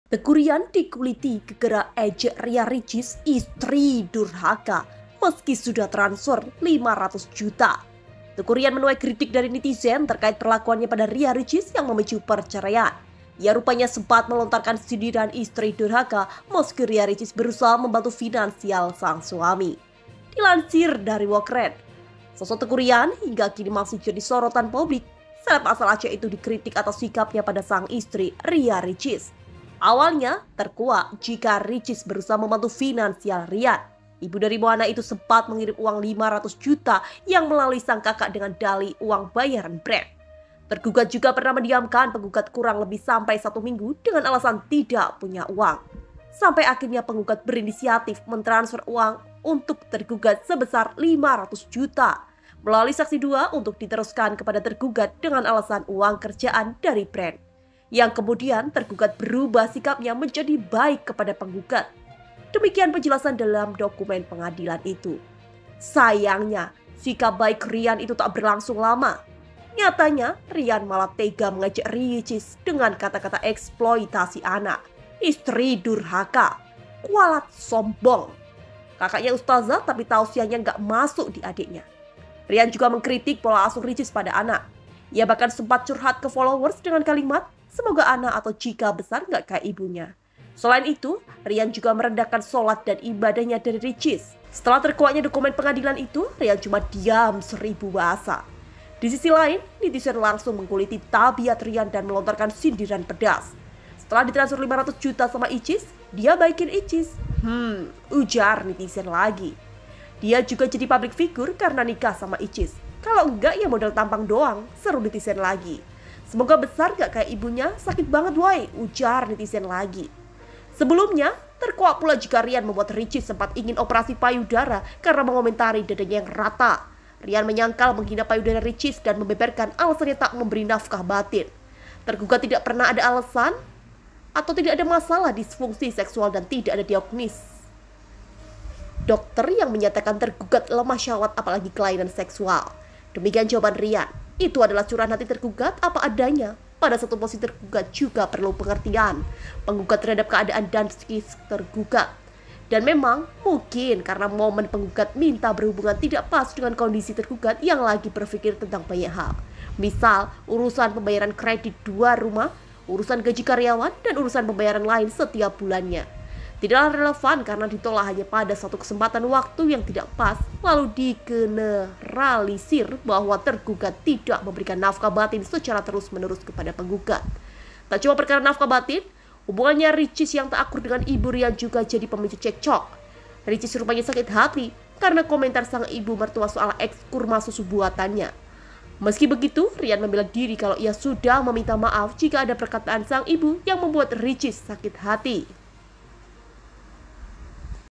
Teuku Ryan Dikuliti Gegara Ejek Ria Ricis Istri Durhaka Meski Sudah Transfer 500 Juta
Teuku Ryan menuai kritik dari netizen terkait perlakuannya pada Ria Ricis yang memicu perceraian. Ia rupanya sempat melontarkan sindiran istri durhaka meski Ria Ricis berusaha membantu finansial sang suami.